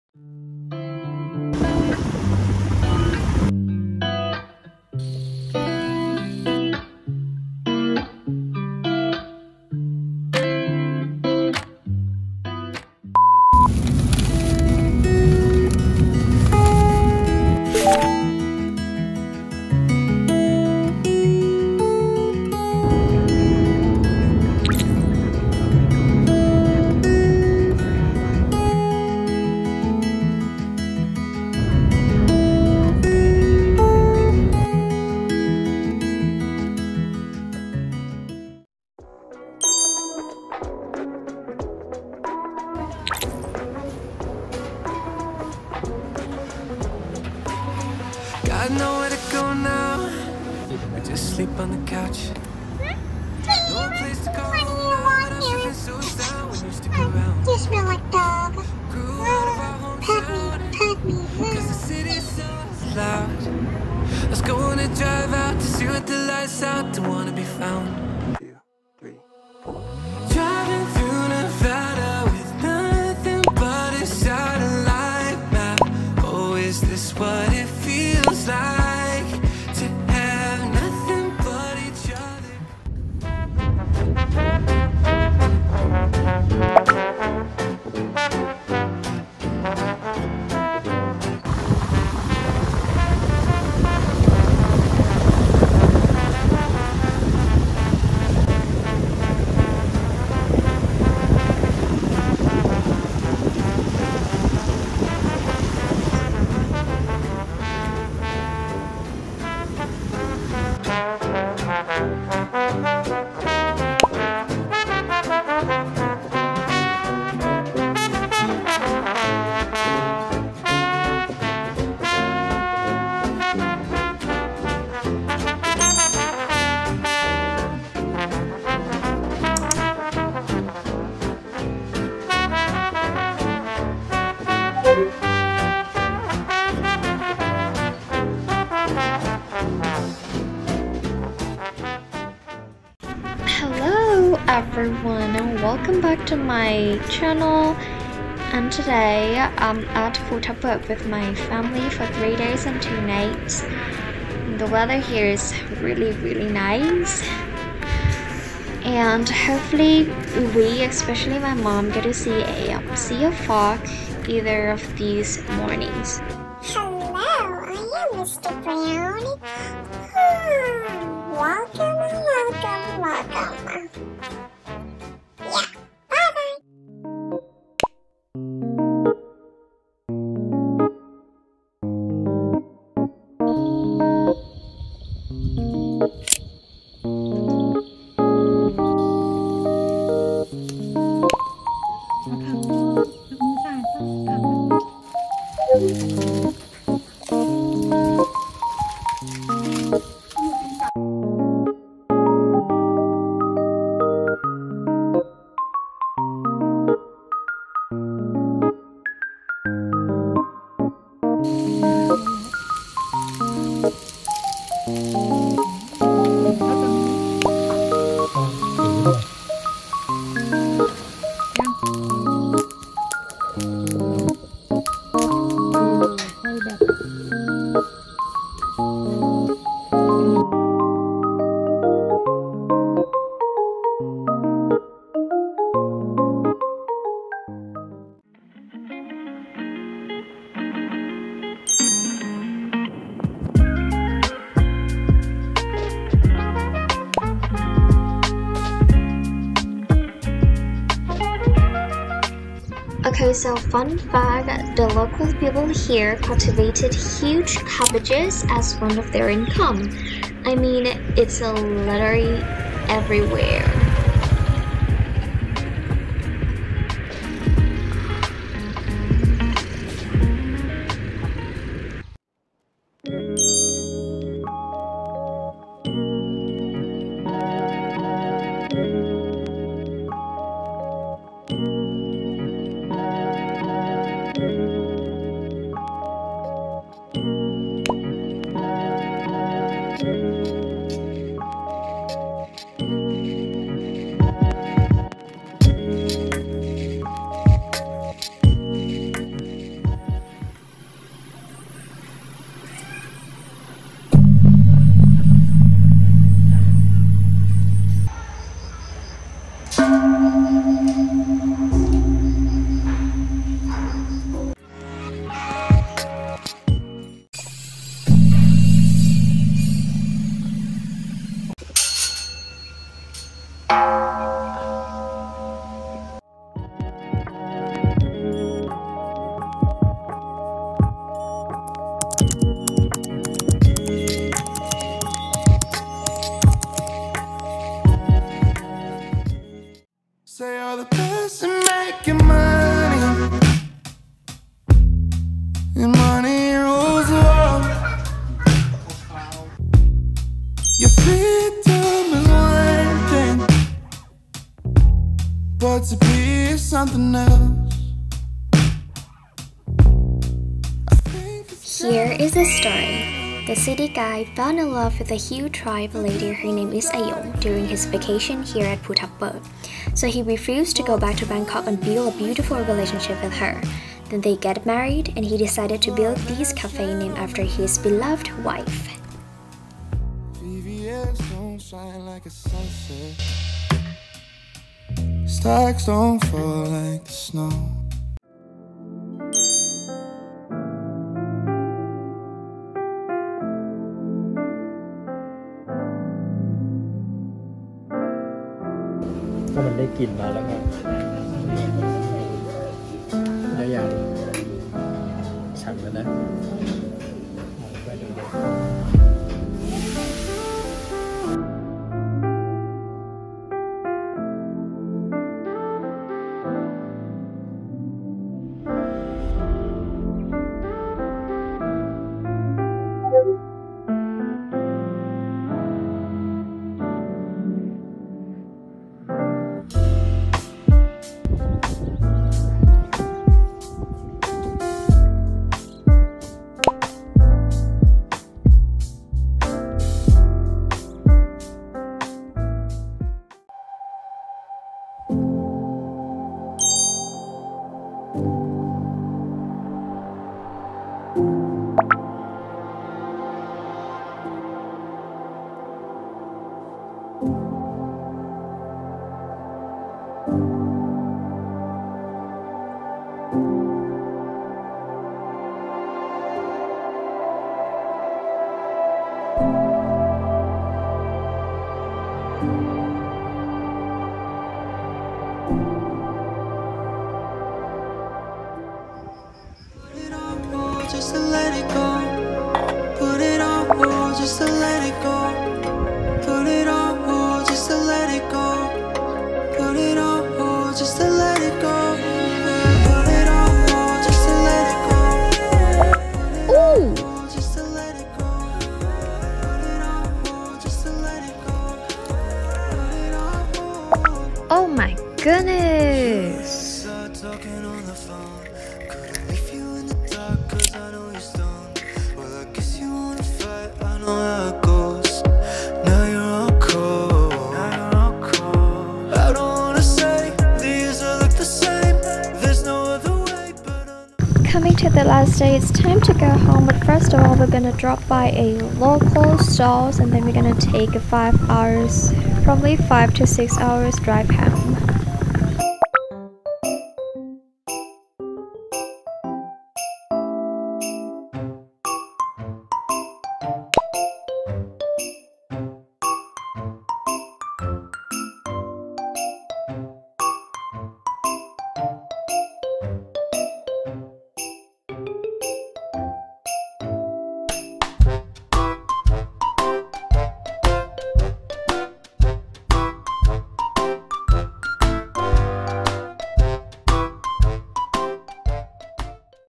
GETAWAY Vlog | Phu Thap Buek (ภูทับเบิก), beautiful landscape and things to do on this mountain
A family trip just to see a beautiful sea of fog💨💨💨 Relax your mind and please enjoy the vibe!🍃

on Jul 20, 2022

And Mixkit